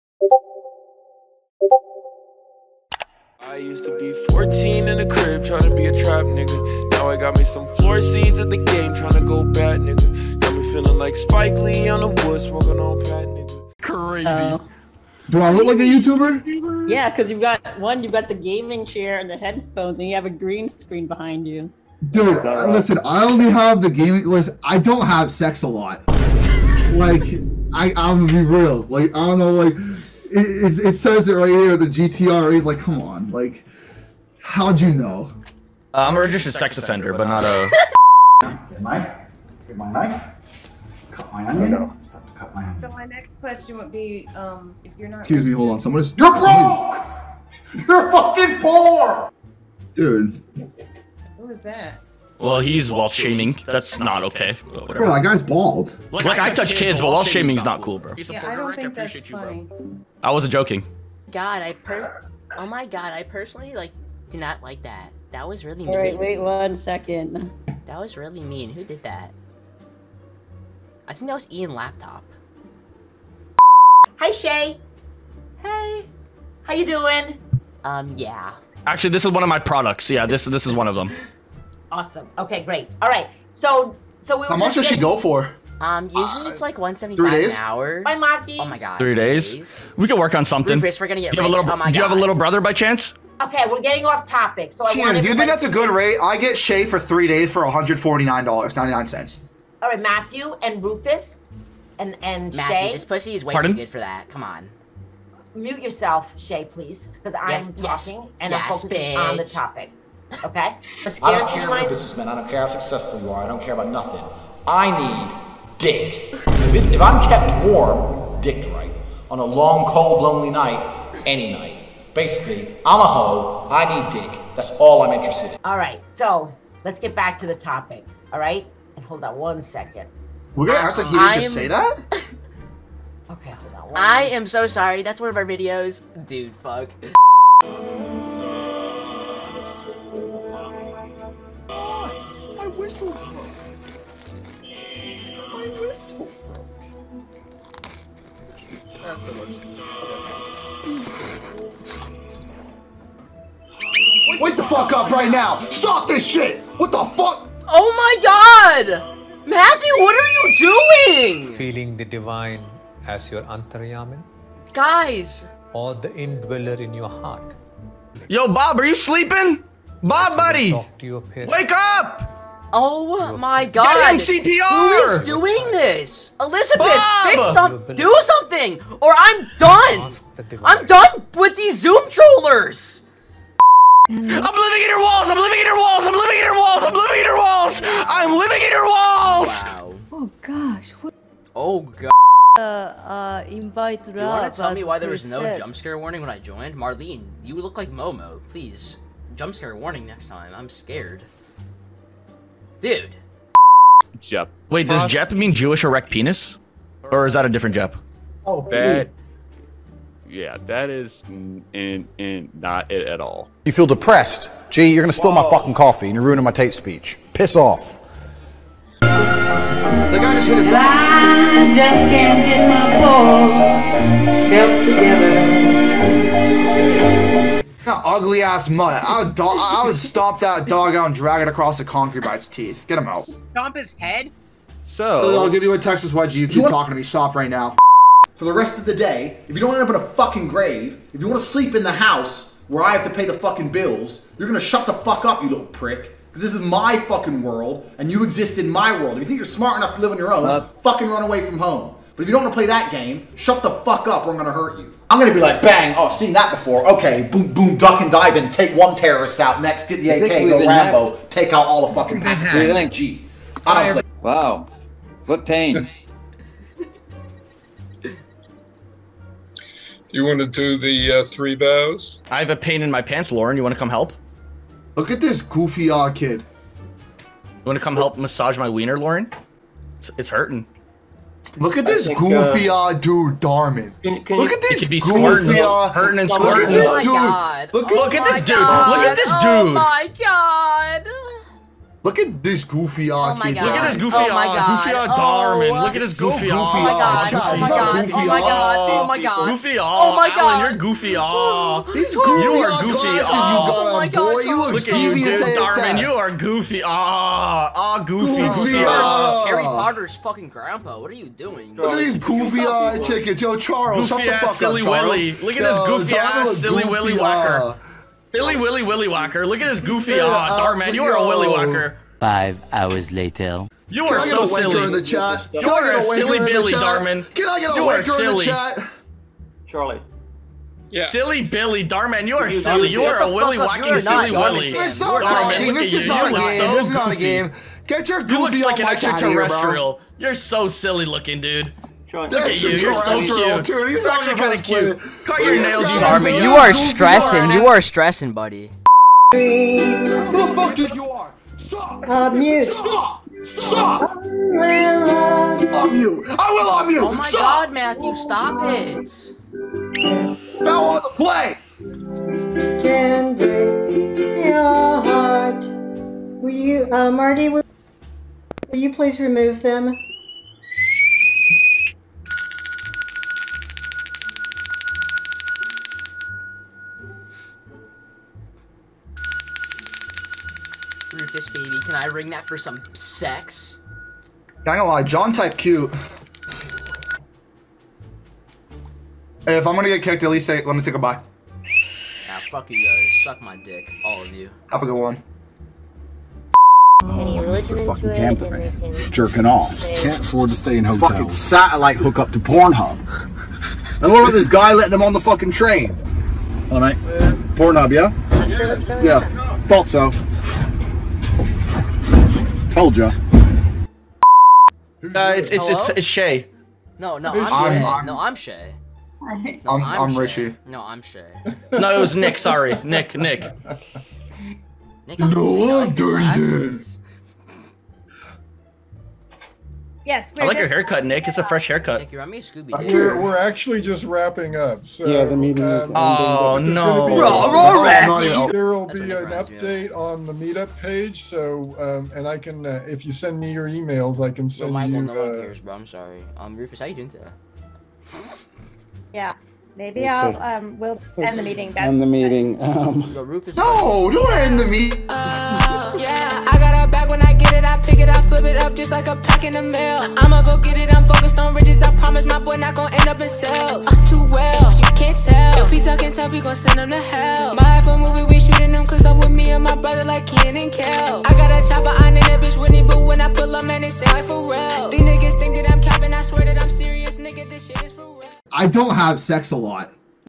Trolling Zoom Classes....But I'm ANDREW TATE!
Today we online zoom bombed karens using Andrew Tate!

Thanks for making it this far! Have an amazing day :)